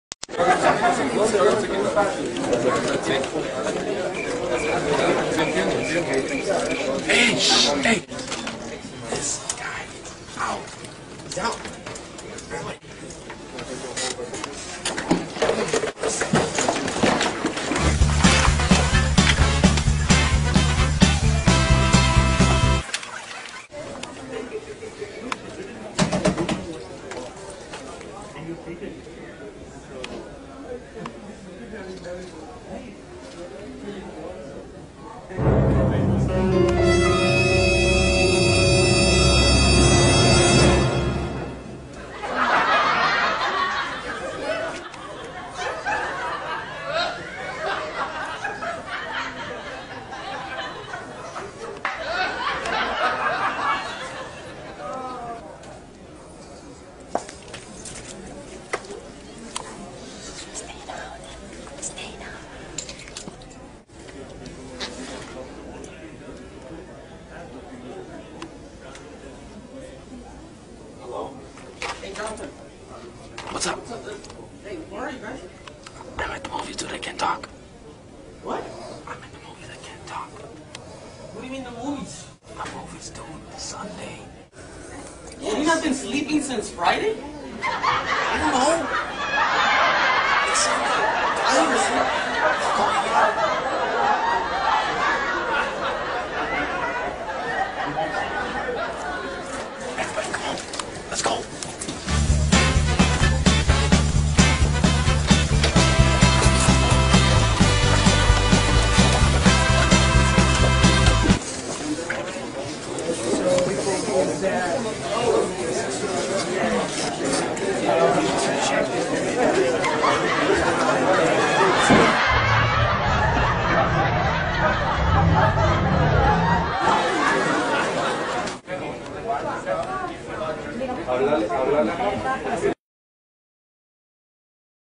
OMG, this is so hilarious! You mean I've been sleeping since Friday??
Do you fall asleep at work? Don't miss this hilarious moment at workplace!